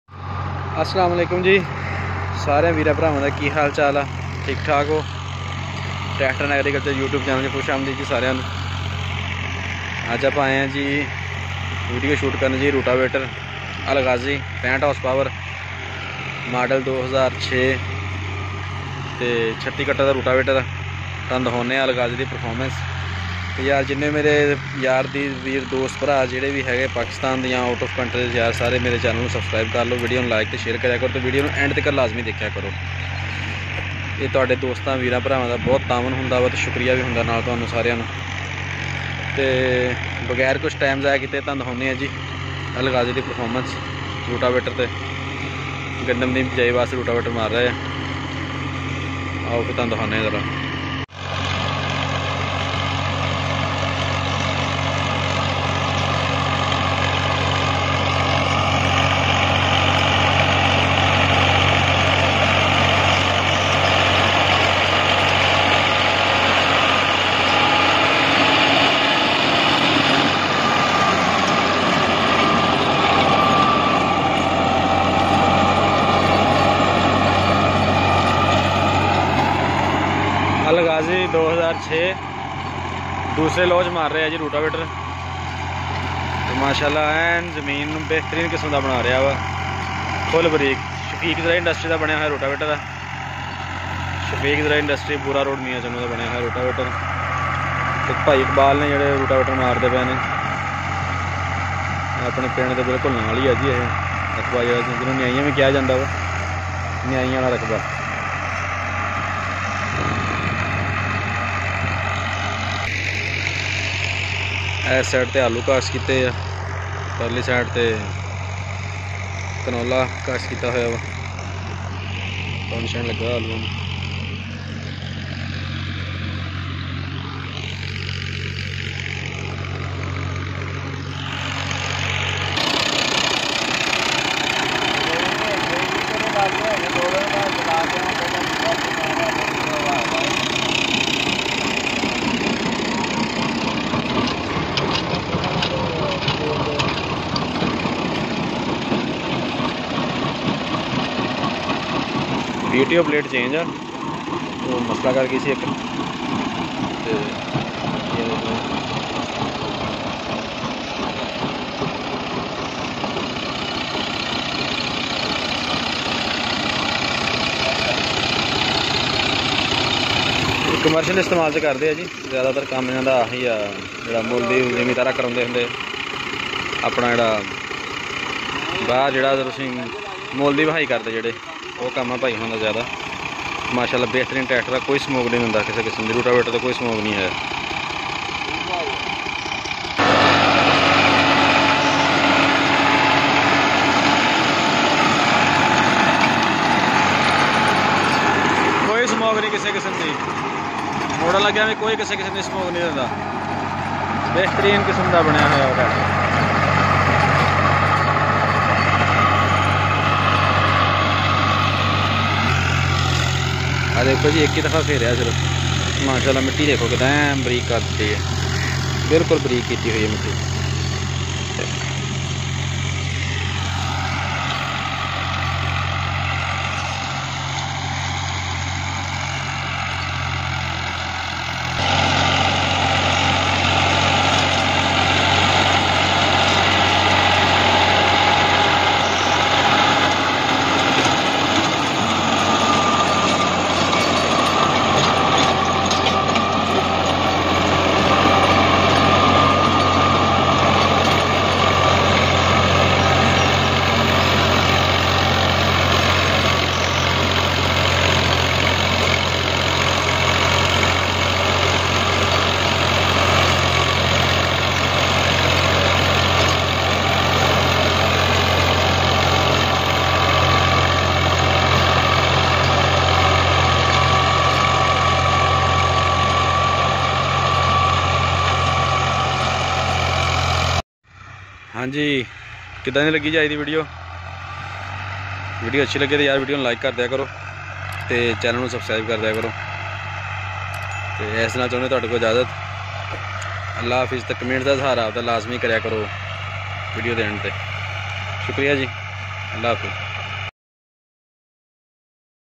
AL GHAZI 65 HP| NEW HOLLAND|| ROTAWAITER PERFORMANCE
hi dear views New Holland Al ghazi very best performance in field rotawaiter is a main implements in field he used in agriculture